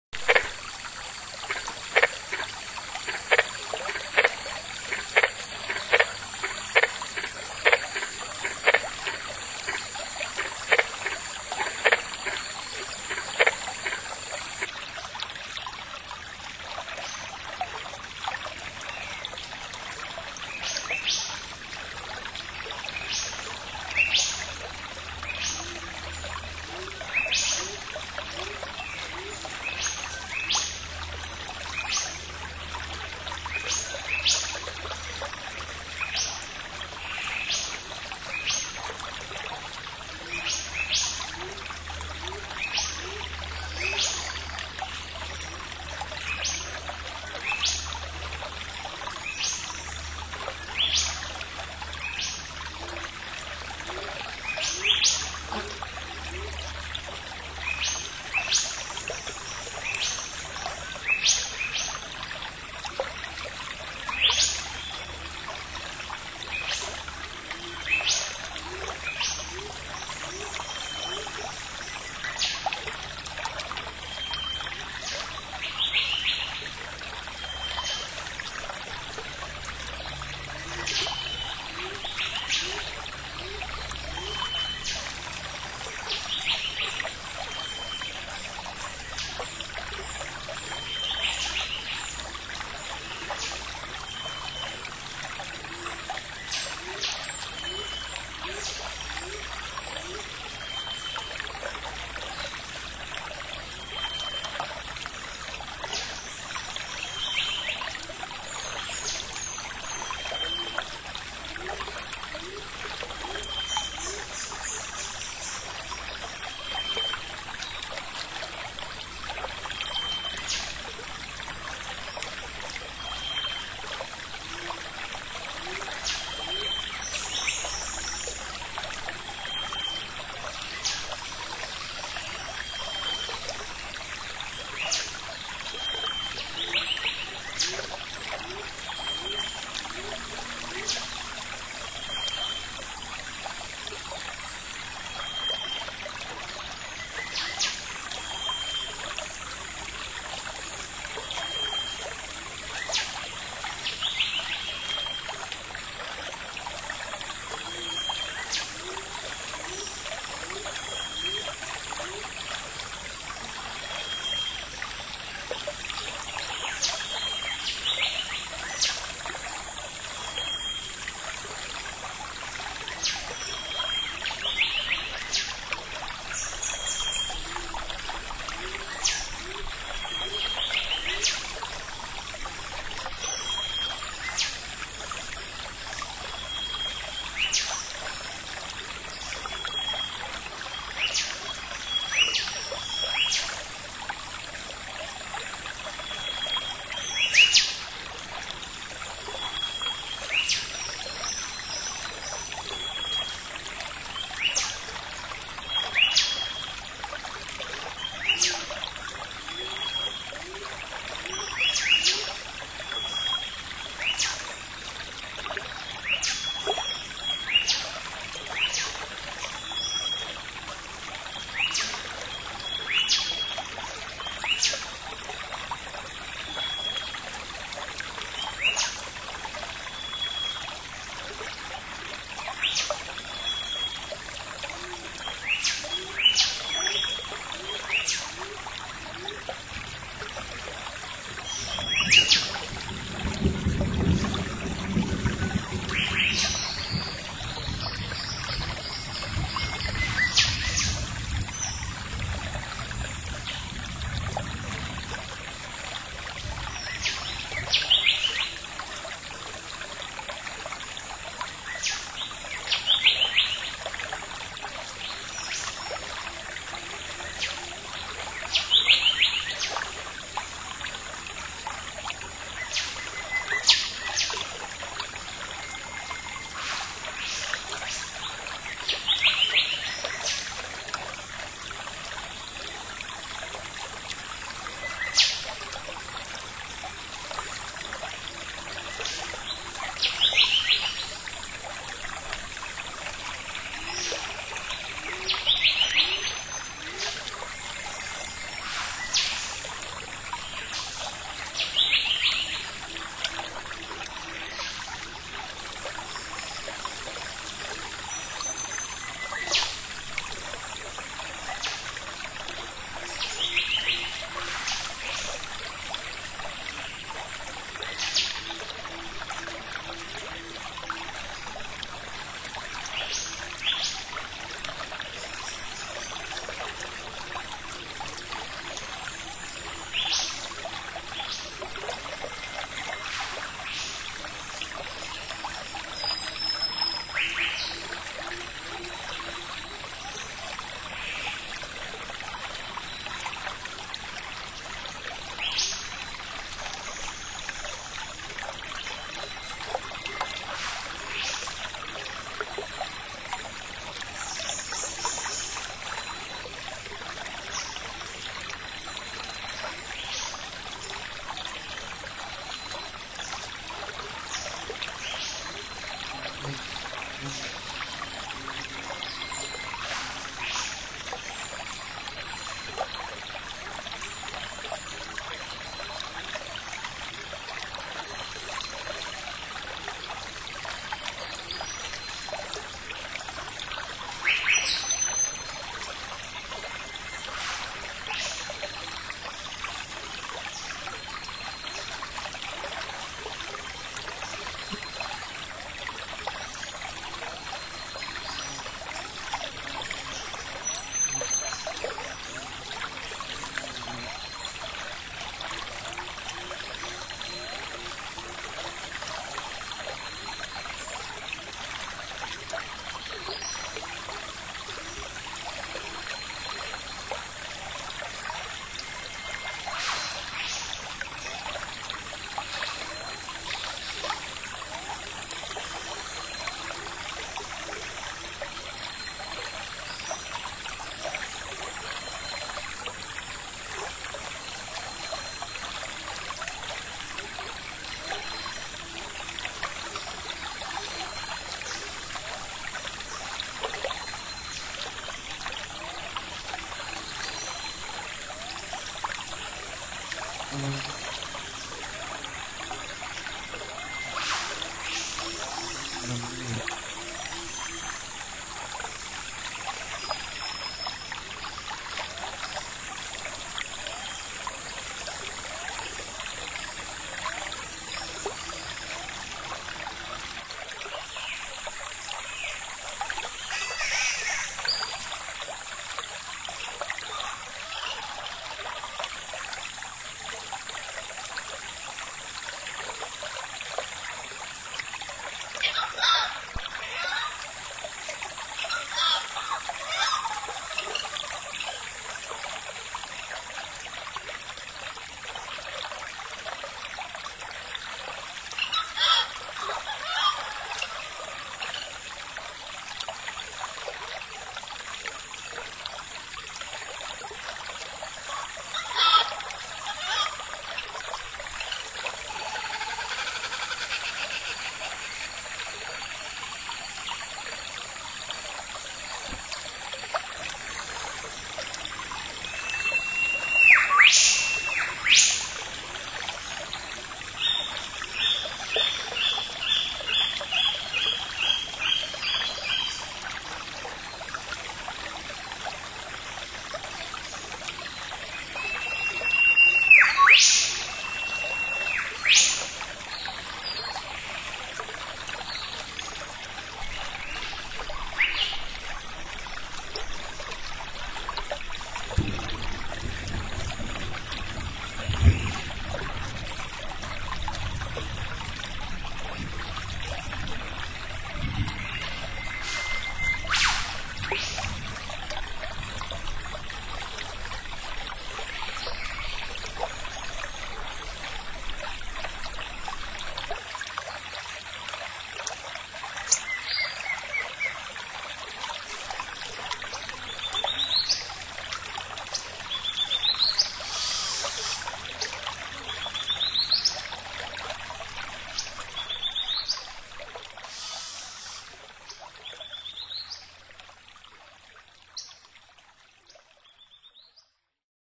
Death of a White-lipped Green Treefrog
Australian White-lipped Green Treefrog (Litoria infrarenata) being eaten by a Green Tree Snake (Dendrelaphis punctulata.)